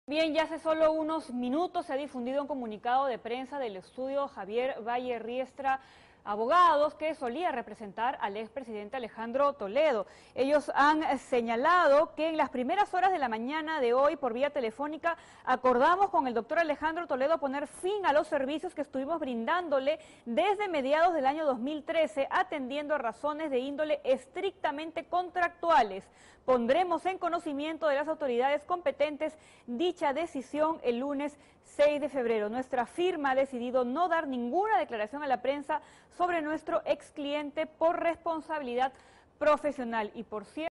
Estudio Javier Valle-Riestra dejó de asesorar a Alejandro Toledo
El estudio Javier Valle-Riestra Abogados informó este viernes que puso fin a los servicios que brindaba al ex presidente Alejandro Toledo, a quien defendió legalmente desde el año 2013. El anuncio lo hizo Luciano López, quien fue abogado del líder de la agrupación política Perú Política.

“En las primeras horas de la mañana de hoy, por vía telefónica, acordamos con el Dr. Alejandro Toledo poner fin a los servicios que estuvimos brindándole desde mediados del año 2013 atendiendo a razones de índole estrictamente contractuales. Pondremos en conocimiento de las autoridades competentes dicha decisión el lunes 6 de febrero”, refiere el comunicado.

Asimismo, el estudio de abogados aseguró que ha decidido no dar ninguna declaración a la prensa sobre su ex cliente por responsabilidad profesional. El documento fue emitido luego que se conociera que Toledo Manrique habría recibido la suma de US$20 millones por parte de la empresa brasileña Odebrecht.

Luego de conocer esta denuncia, desde París, Francia, Alejandro Toledo se comunicó vía telefónica con el diario El Comercio y negó tajantemente que haya recibido dinero de la constructora brasileña. El ex mandatario de la República aseguró que todas estas acusaciones responden a una persecución política.

2